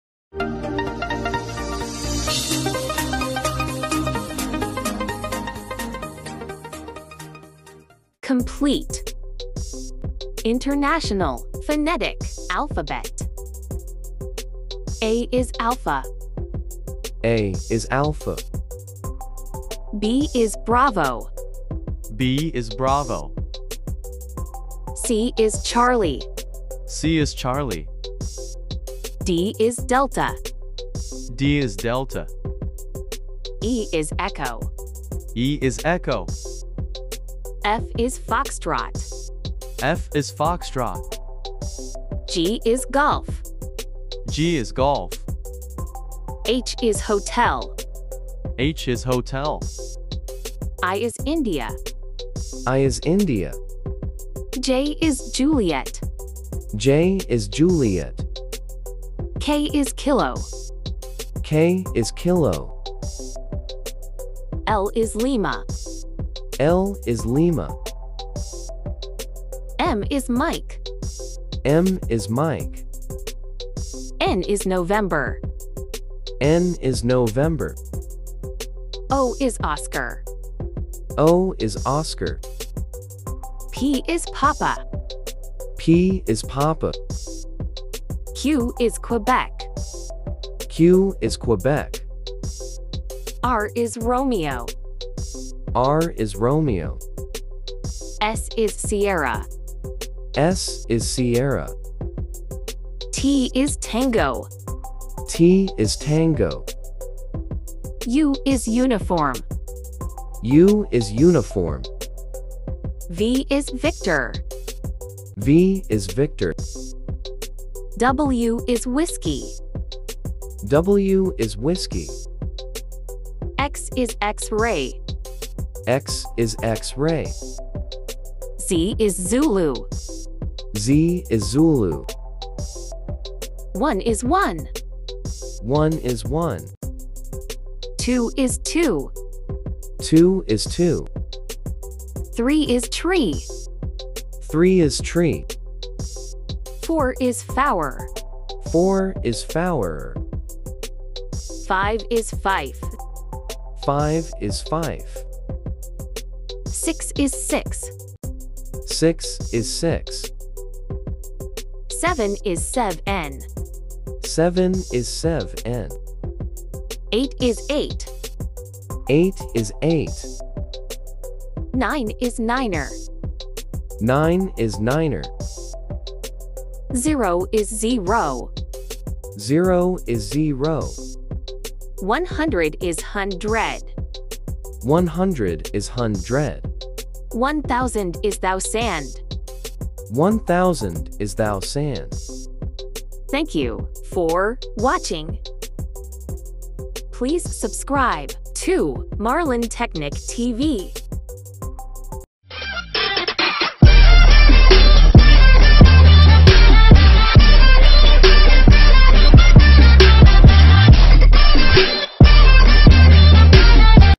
Updated NATO/ICAO international Phonetic Alphabet and Numbers 2022
This is the latest and updated International Phonetic Alphabet and Numbers widely used in Aviation Industry recognized by NATO - North Atlantic Treaty Organization and ICAO - International Civil Aviation Organization.

Related Search

International Phonetic Alphabet
Call Center pronunciation
FAA
EASA
A350
A330
A320
B777
B747
B737
ATA Chapter
NATO
ICAO
Boy Scout
Girl Scout